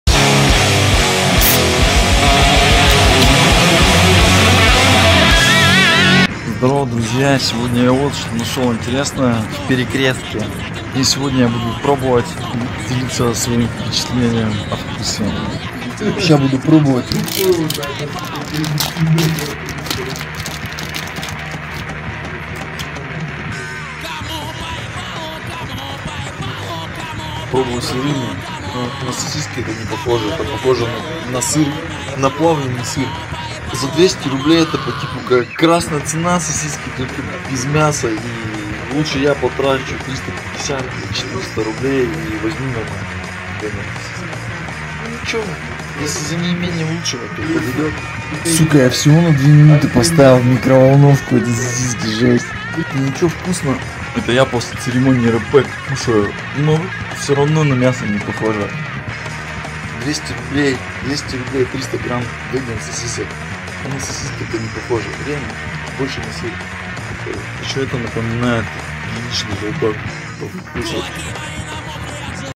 пробуем сосиски от НЕ МЯСО из Перика | стоит ли?
короче можно было крем пасту из них сделать)